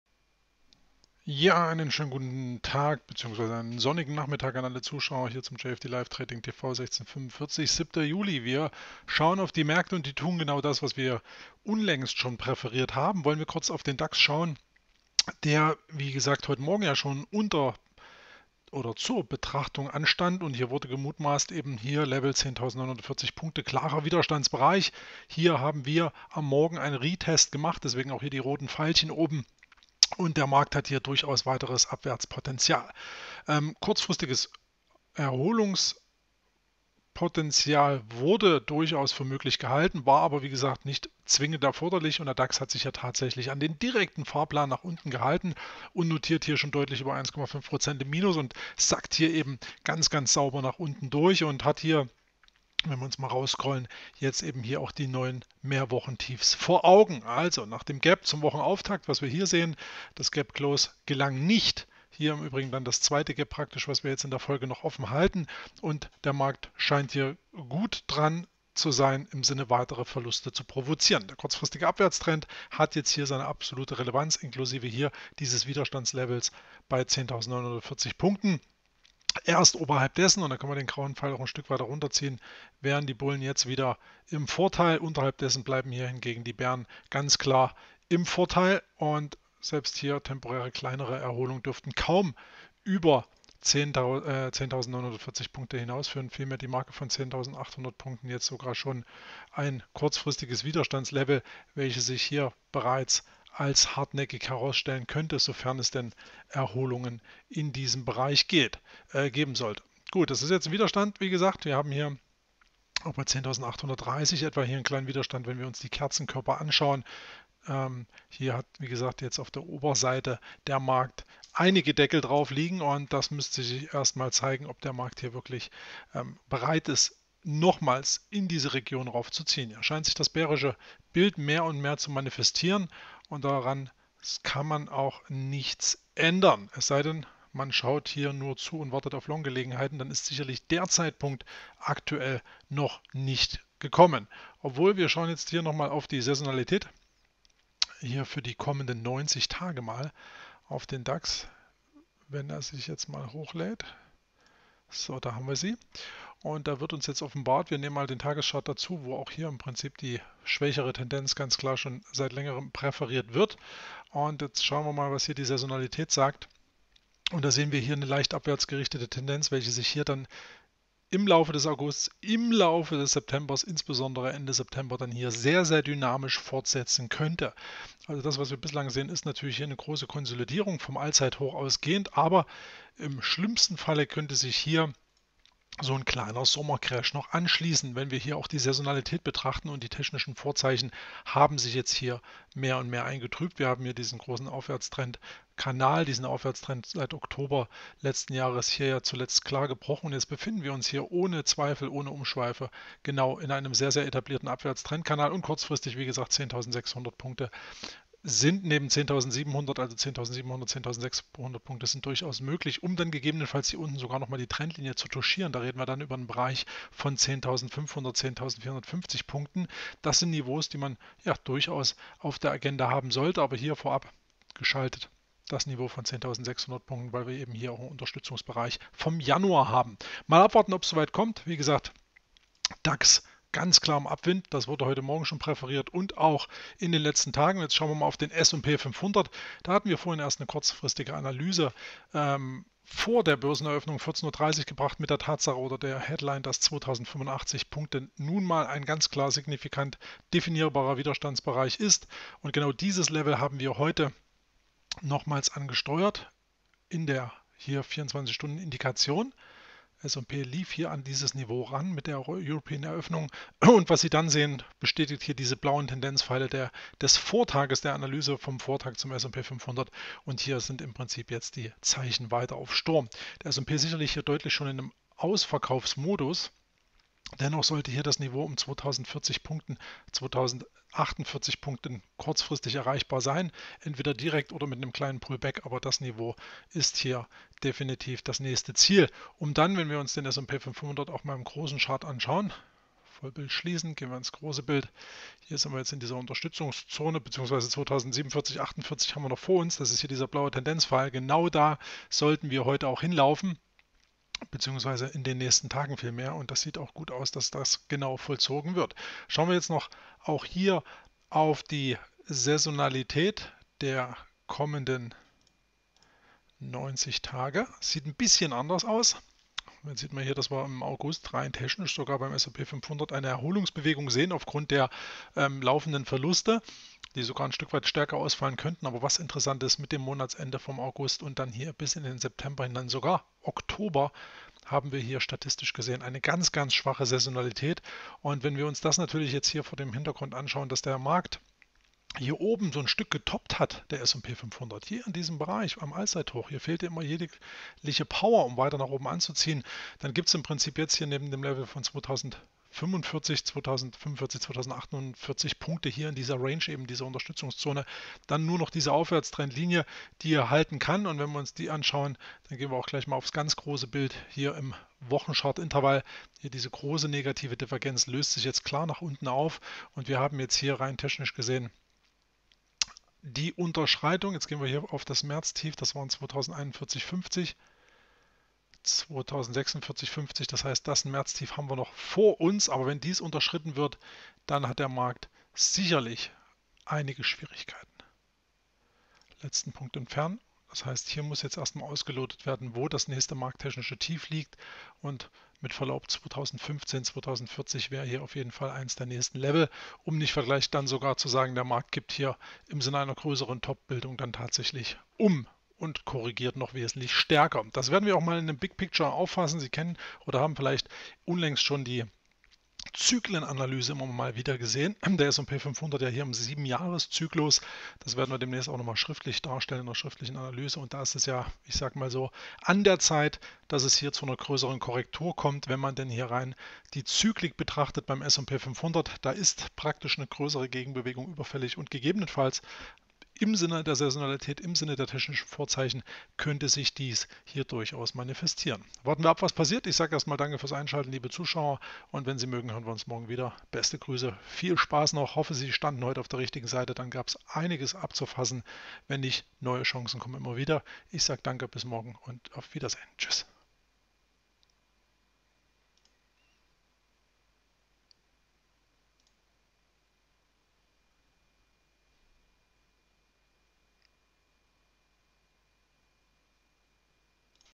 DAX- und S&P 500-Verluste auf breiter Front – doch keine Überraschung für SIE!
Die Indizes (DAX und S&P 500) begeben sich auf Talfahrt. Die technischen Tendenzen waren zuvor allerdings selten so einheitlich und somit war dies keine Überraschung für Sie als Zuschauer der täglichen Sendungen.